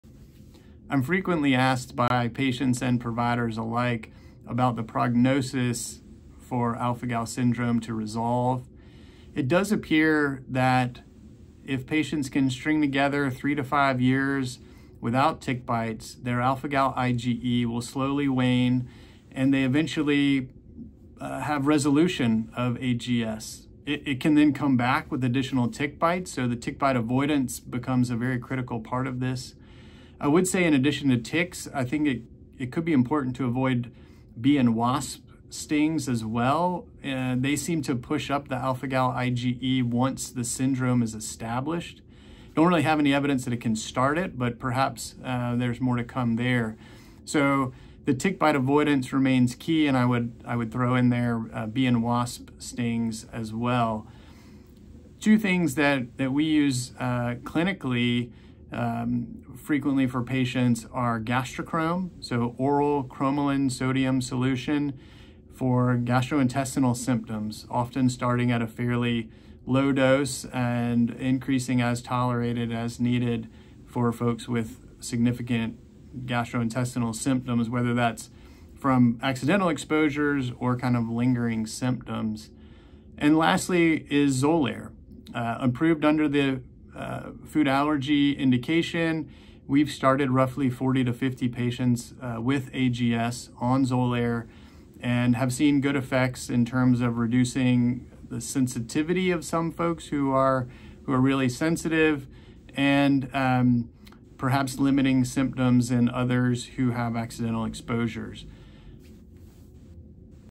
Alpha-gal syndrome prognosis, cromolyn and Xolair: pearls from expert Scott Commins, MD, PhD
Expert Scott Commins, MD, PhD discusses pearls related to alpha-gal syndrome prognosis and the use of cromolyn and Xolair in the diagnosis of AGS.